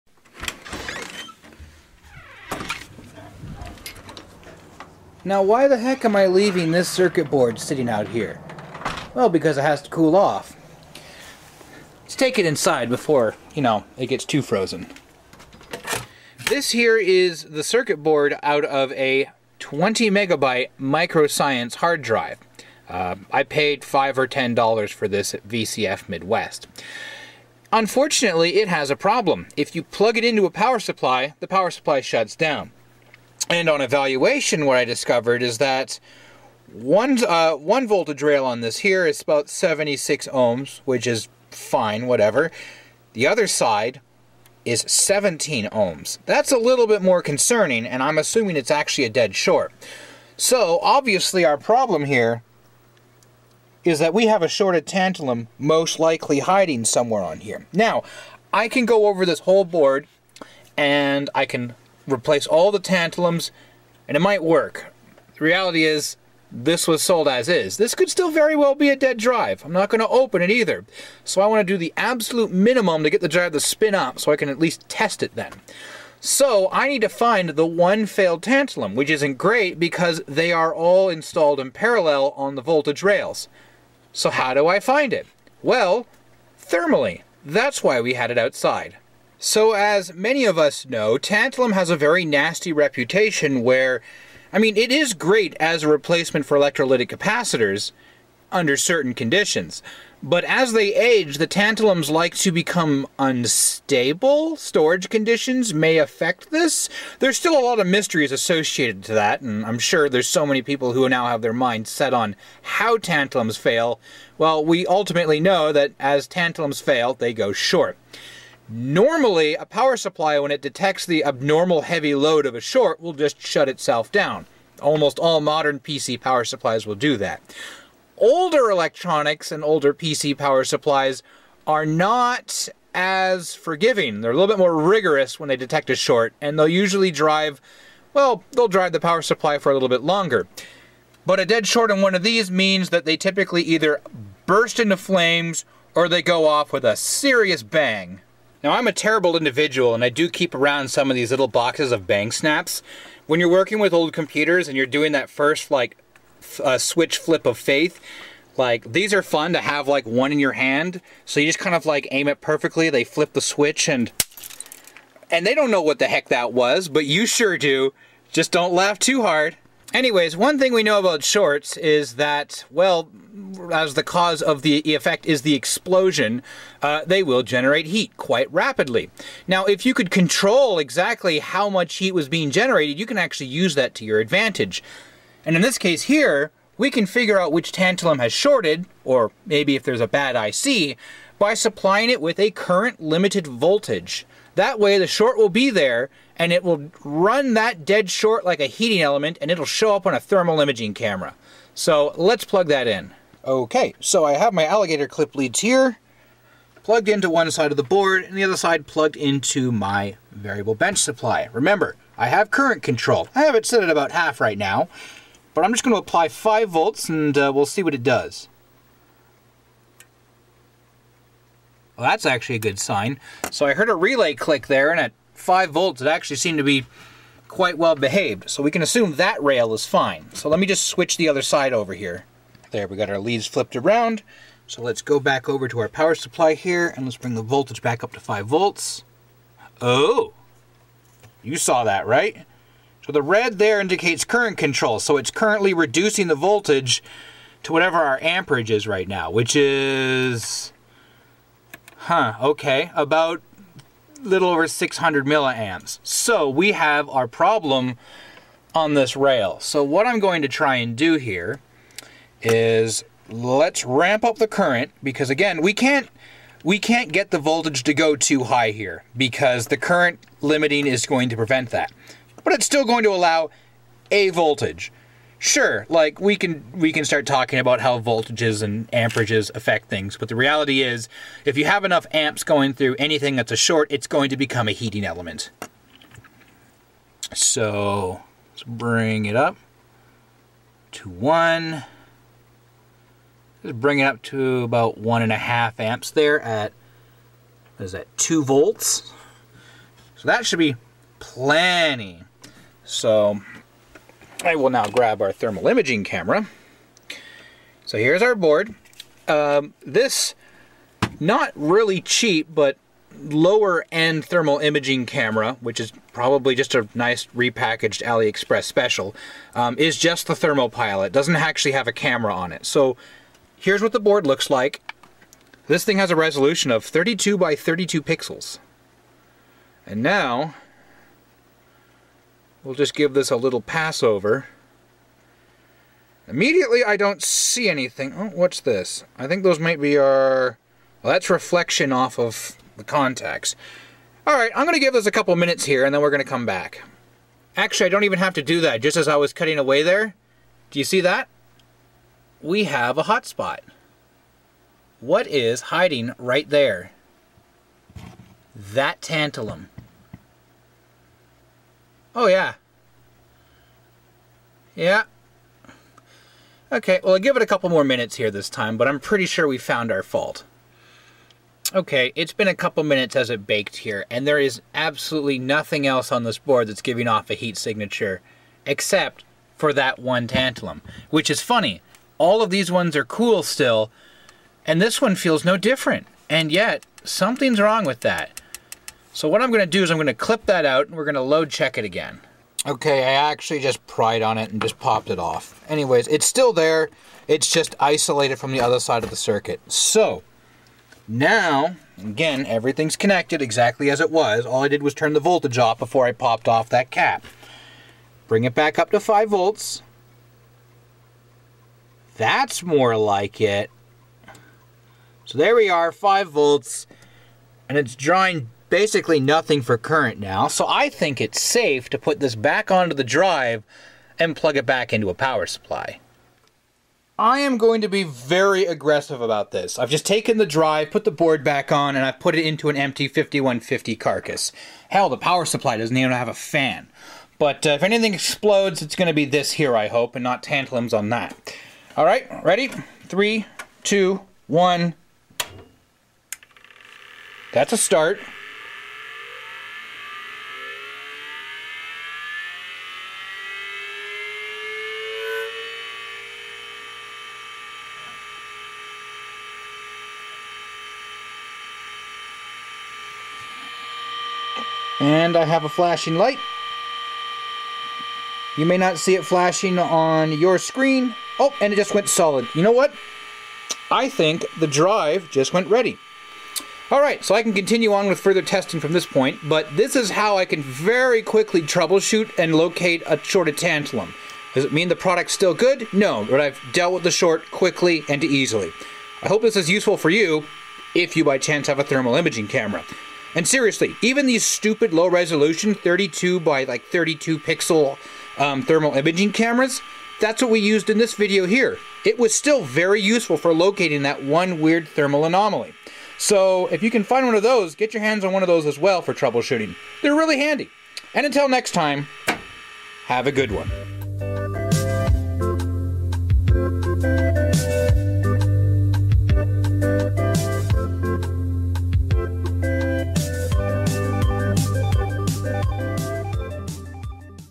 Tantalum hunting with a Thermal Imaging Camera
Tantalum is never fun to deal with because when it shorts and DOESN'T explode you end up with a whole bunch of them running parallel in the same circuit. With the ever dropping price of thermal imaging cameras however using only a current limiting bench supply we can easily isolate the  problem capacitor. Let me show you how using this hard drive from an IBM 5150.

Remember to follow me on "X" at @CelGenStudios to keep up to date on what I am doing and what might be happening in the next video.